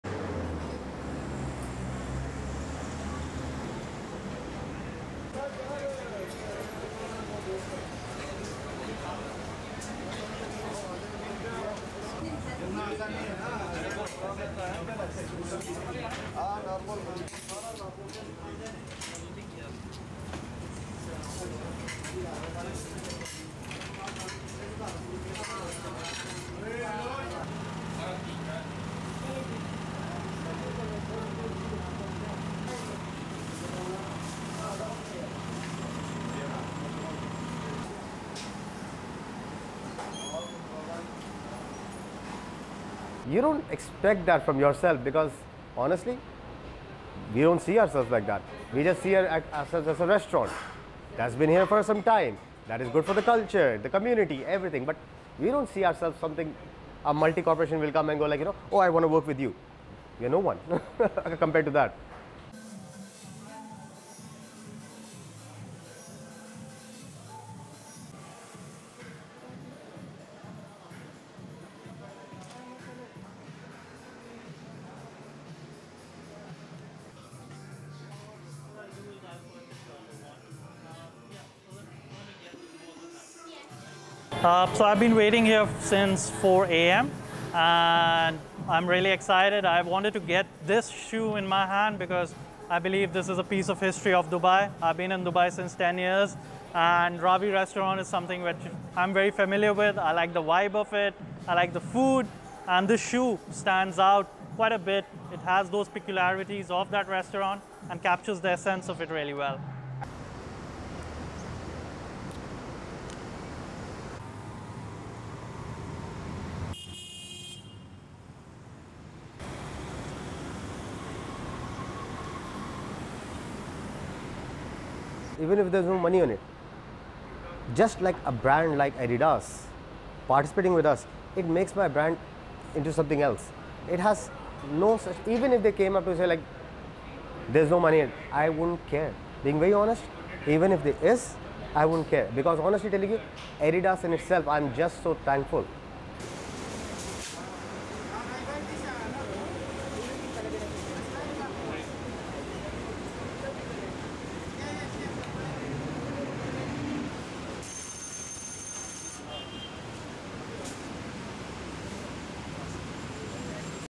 Pakistani restaurant stands test of time in Dubai
(30 Jun 2022) UAE PAKISTANI RESTAURANT

RESTRICTION SUMMARY:
LENGTH: 2:43
ASSOCIATED PRESS
Dubai - 22 June 2022
++NIGHT SHOTS++
1. Various of people sitting at Ravi's, eating
2. Employees' shoes
3. Various of photo of Ravi employee holding Superstar Ravi sneaker
4. Photo of Ravi employees wearing Superstar Ravi sneakers
5. SOUNDBITE (English) Waseem Abdul Hameed, Ravi restaurant operations manager:
"You don't expect that from yourself because honestly, we don't see ourselves like that. We just see ourselves as a restaurant that's been here for some time, that is good for the culture, for the community, everything, but we don't see ourselves as something a multi-corporation will come to and go like 'oh I want to work with you.' We're no one compared to that."
ASSOCIATED PRESS
Dubai - 23 June 2022
6. Various Superstar Ravi sneaker
7. People standing in line at Adidas store
8. Man buying shoes
9. SOUNDBITE (English) Tooshank Sharma, Dubai resident:
"So I've been waiting here since 4:00 a.m. and I'm really excited. I wanted to get this shoe in my hand because I believe this is a piece of history of Dubai. I've been in Dubai since 10 years and Ravi restaurant is something that I'm very familiar with, I like the vibe of it, I like the food, and the shoe stands out quite a bit. It has those peculiarities of the restaurant and captures their sense of it really well."
ASSOCIATED PRESS
10 . Exterior of Ravi restaurant
11. Various of Ravi X Adidas truck
12. SOUNDBITE (English) Waseem Abdul Hameed, Ravi restaurant operations manager:
"Even if there's no money in it, just like a brand like Adidas participating with us, it makes my brand into something else. Even if they came and said there's no money in it, I wouldn't care. Being very honest; even if there is, I wouldn't care. Because honestly telling you, Adidas in itself, I'm just so thankful."
13. Various of men taking pictures by Ravi X Adidas truck and Adidas sign ++NIGHT SHOTS++
LEADIN:
In a city dotted with sea-view brunches and endless high-end Asian food options, a small hole-in-the-wall restaurant with plastic chairs and metal tables continues to be the go-to spot for many of Dubai's residents.
STORYLINE:
Ravi restaurant has become a staple of Dubai's food culture, a place where the majority of the Emirate's residents have been when they're craving a hearty meal or after a night out.
For people in the UAE, it's a nostalgic spot.
The restaurant opened its doors in the United Arab Emirates in 1978 in Satwa, at a time when it was a sandy area full of big lorries and small construction shops.
Today, Ravi is constantly bustling with hungry customers wearing everything from business suits to traditional saris or sneakers.
The smell of bread, butter chicken, and barbecue fills the air, as the sound of the ceiling fans is drowned out by the chatter of the people.
It constantly features on the list of must-see attractions in Dubai.
Ravi is seen as such an authentic cornerstone of Dubai, so much so that Adidas approached Ravi for brand sponsorship.
The collaboration launched just last week; a limited edition green-striped sneaker called the Superstar Ravi.
On the heel tab is the year Ravi opened and its name in both Arabic and English on both shoes.
On the inside of the tongue is a menu with a list of six of the most famous dishes at Ravi's that the owners selected.
Ravi for many who know Dubai is as authentic as it gets in a country known for its glitz and glamour.